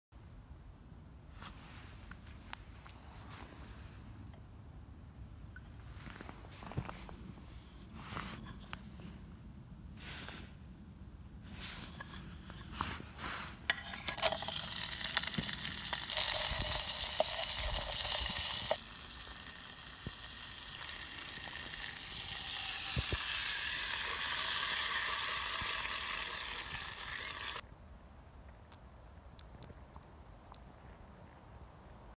OUTDOOR COOKING - Chorizo & Beans w/ HOBO
Chorizo und Baked Beans mit dem Hobo gekocht!